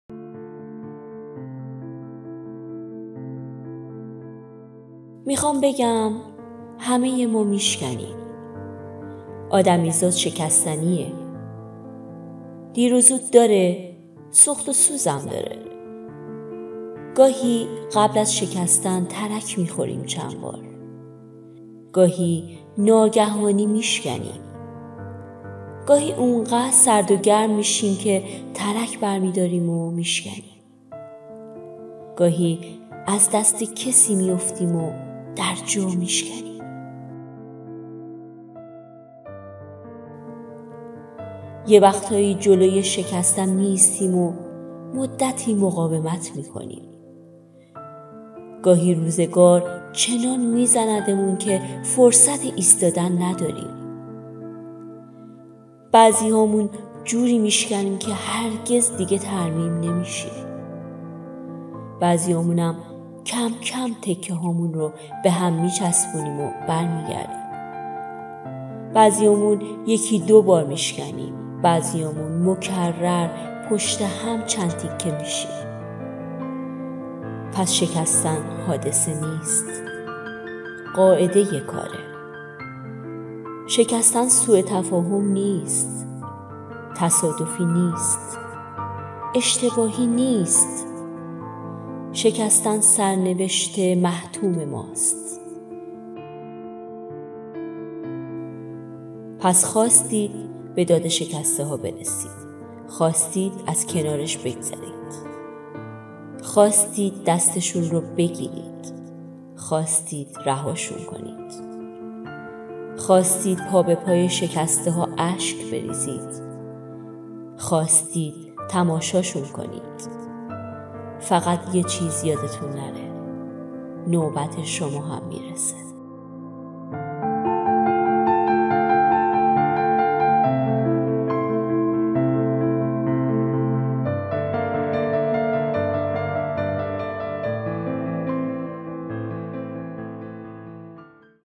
انگیزشی همه ما آدما میشکنیم پس بیایم به داد شکسته ها برسیم و بی تفاوت از کنارشون رد نشیم
سابسکرایب و لایک فراموش نشه و اگه اون زنگوله رو هم بزنید به محض اینکه ویدیوی جدید آپلود کنم شما اولین نفر باخبر خواهید شد
Performed by: Noushin Rabiee